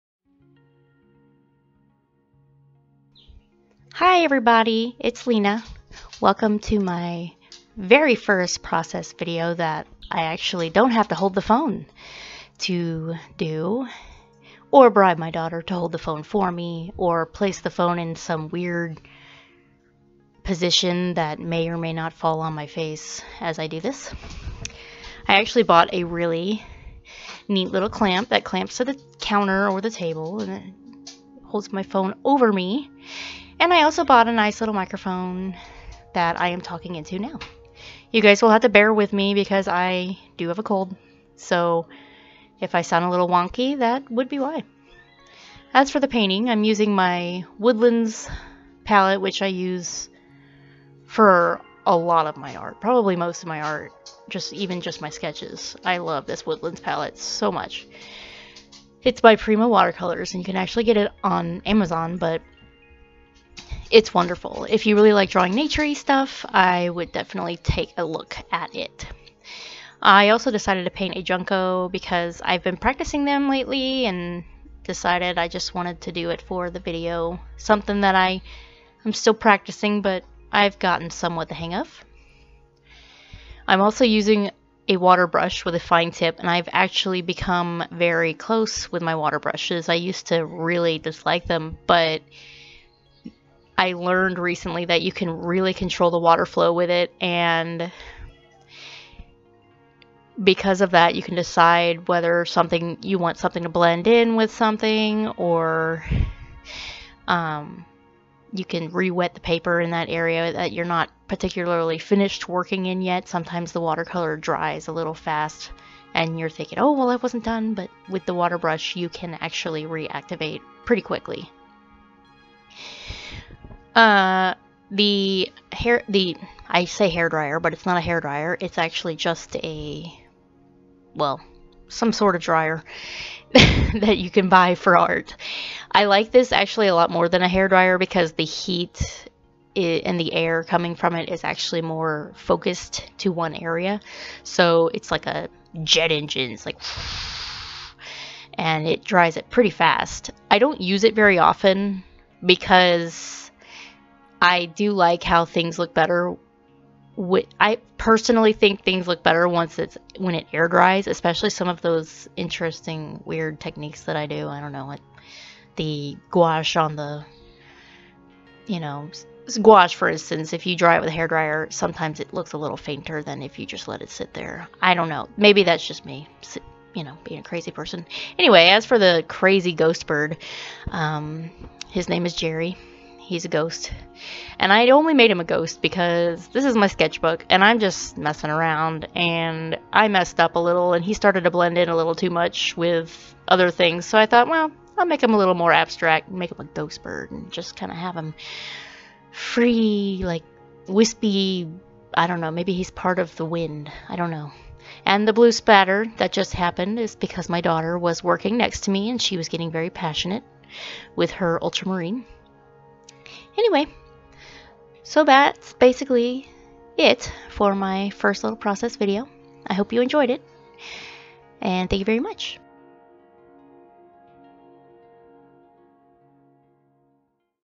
Jerry the Ghost Junco
A process video of my "ghost" junco, Jerry. I used Woodlands by Prima Watercolor Confections and a water brush.